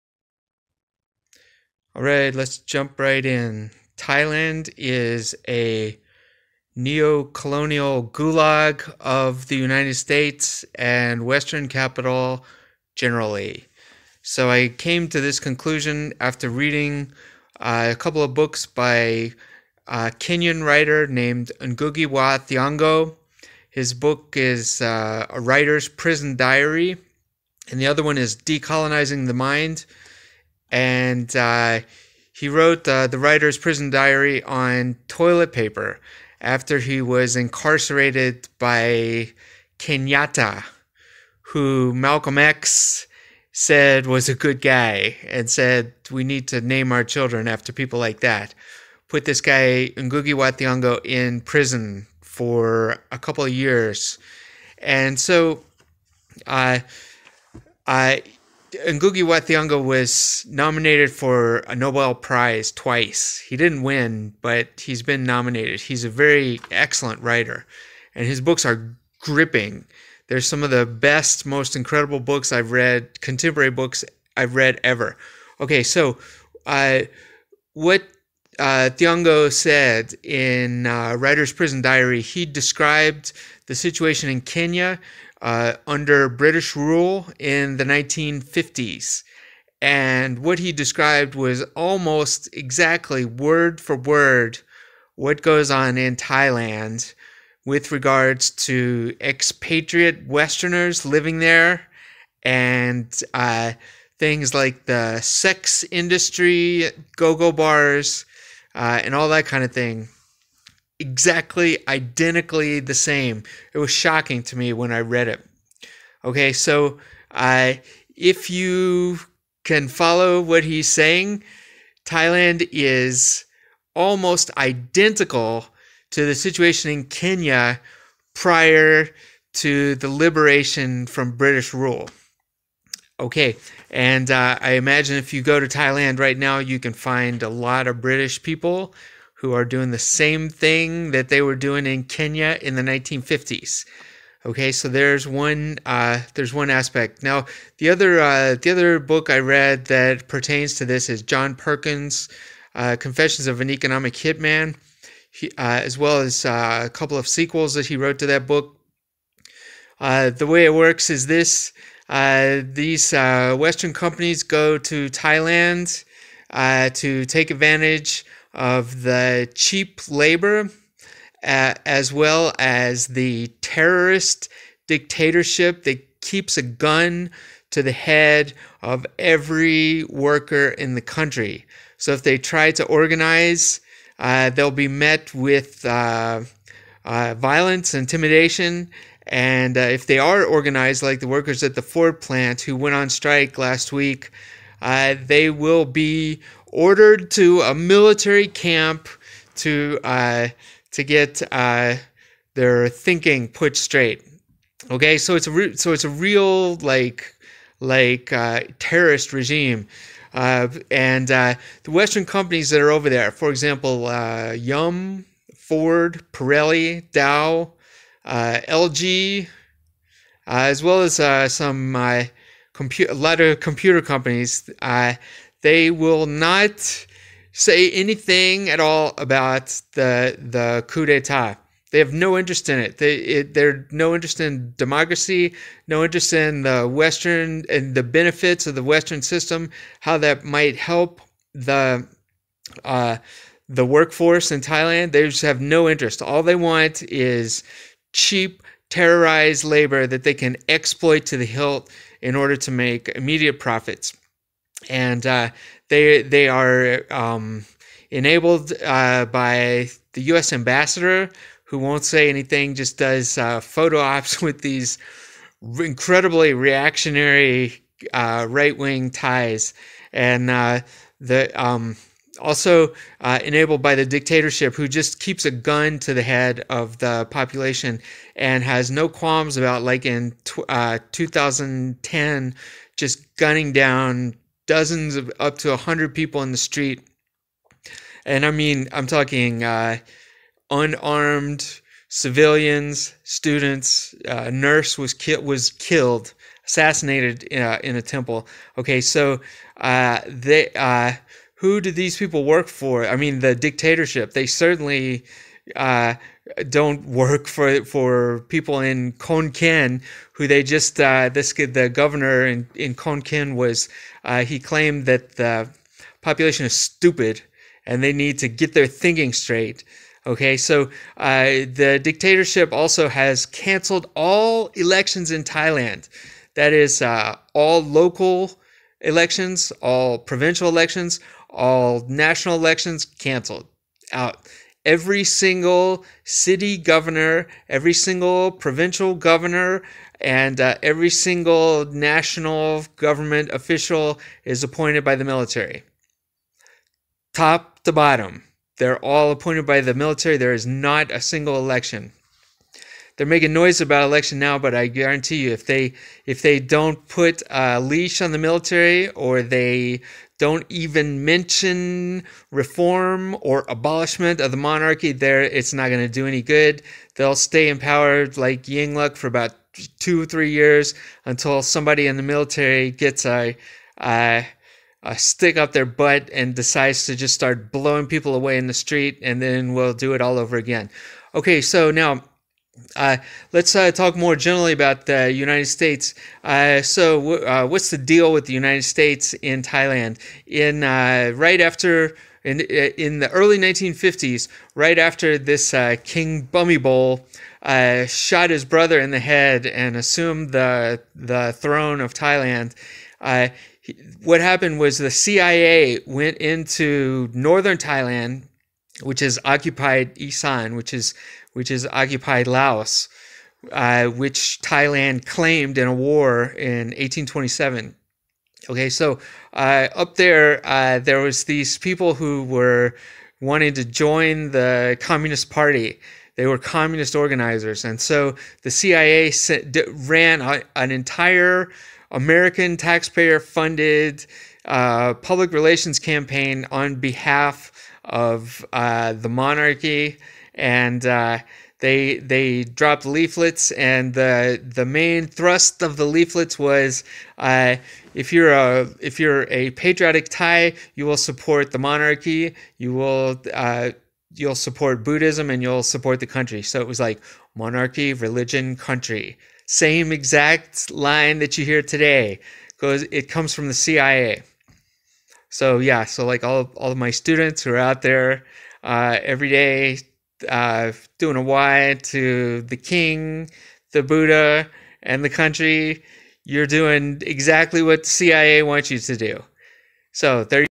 How Thailand is a NeoColonial Gulag of the US and Western Capital part 1
How Thailand is a NeoColonial Gulag of the US and Western Capital